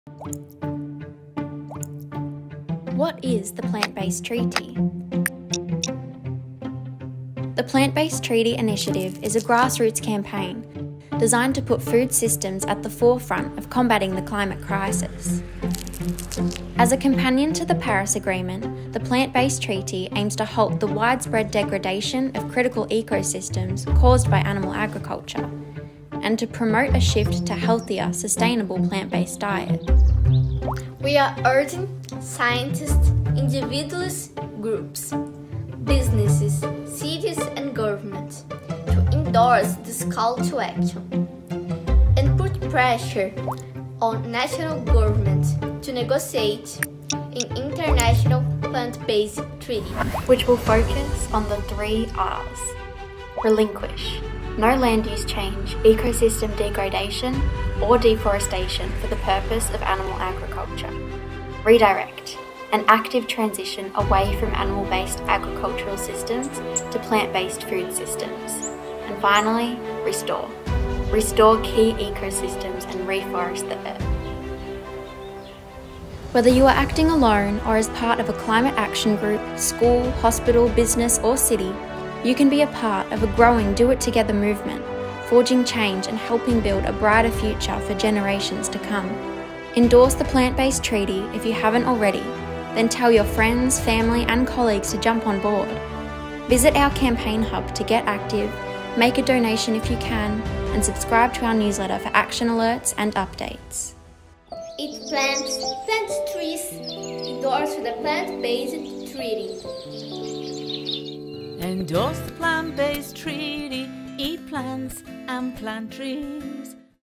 What Is The Plant Based Treaty?
As a companion to the UNFCCC/Paris Agreement, the Plant Based Treaty is a landmark international treaty and first of its kind to put food systems at the heart of combating the climate crisis. The Treaty aims to halt the widespread degradation of critical ecosystems caused by animal agriculture, to promote a shift to more healthy, sustainable plant-based diets and to actively reverse damage done to planetary functions, ecosystem services and biodiversity.

The Treaty urges leaders to negotiate a global agreement around the following three principles:

RELINQUISH - No land use change, ecosystem degradation or deforestation for the purposes of animal agriculture
REDIRECT - An active transition away from animal-based agricultural systems to plant-based food systems
RESTORE - Restore key ecosystems and reforest the Earth